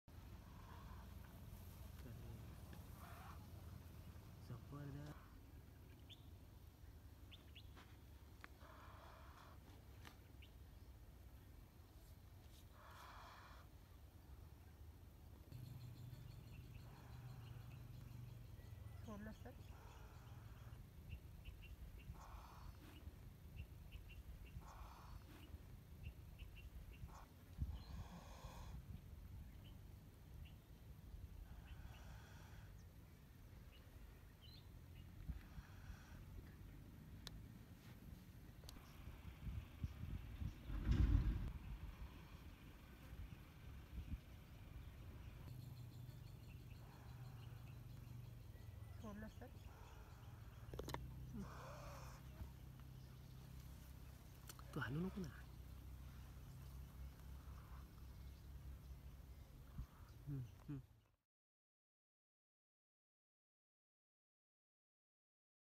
Indian Rat Snake or Dhaman agitated with handling shows off forked tongue
An Indian Rat Snake or Dhaman is agitated with its handling and wriggles and shows off its forked tongue. The distressed snake quickly slithers away once it is loosened from the hand grip.